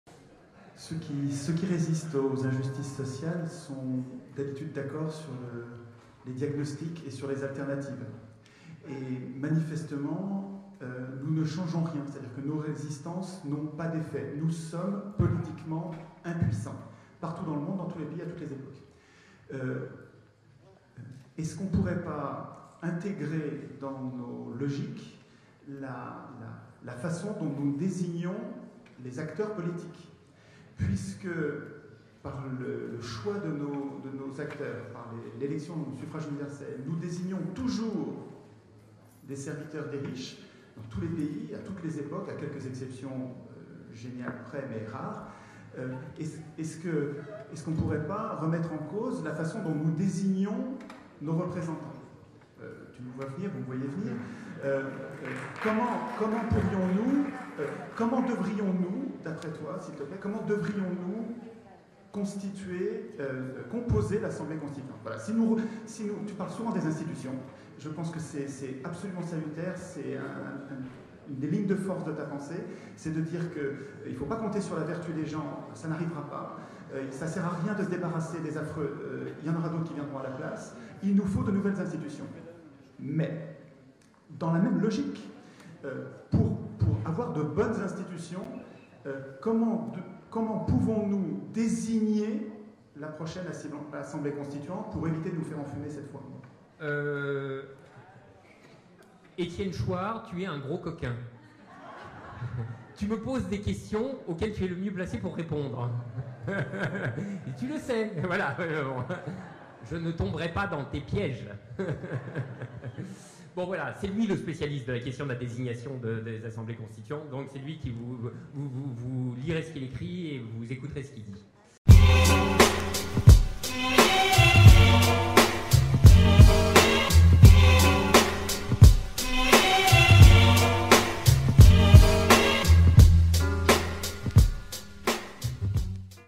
Frederic Lordon répond à Etienne Chouard sur le processus constituant
L'impuissance politique du peuple démontre que nous ne sommes pas en démocratie.
Parce que ce n'est pas aux hommes au pouvoir d'écrire les règles du pouvoir.

Les Gentils Virus : pour réfléchir et propager le Message de la vraie démocratie